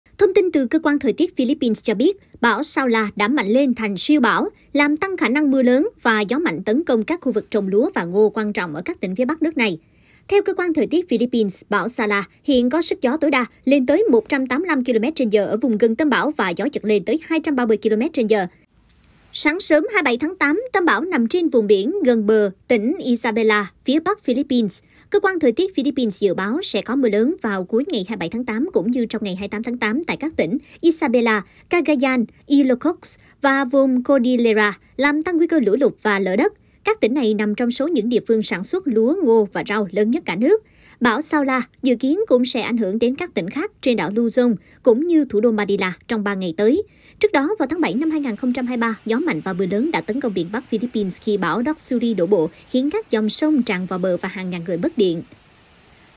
Bão Saola mạnh lên thành siêu bão
Ngày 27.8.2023, thông tin từ Cơ quan thời tiết Philippines cho biết bão Saola đã mạnh lên thành siêu bão, làm tăng khả năng mưa lớn và gió mạnh tấn công các khu vực trồng lúa và ngô quan trọng ở các tỉnh phía bắc nước này.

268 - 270 Nguyễn Đình Chiểu, Phường Võ Thị Sáu, Q.3, TP HCM